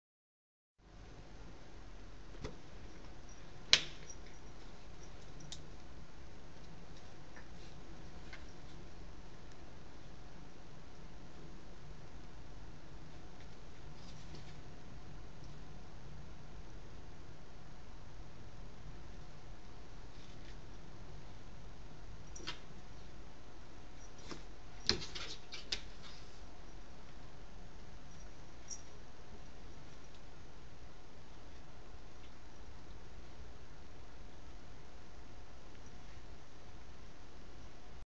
Miles vs. a Humming Bird - 8.11.12
Miles takes on one of Carol's hand-made humming birds!  He loved it, and your cats should too!  Please check out Carol's great items for sale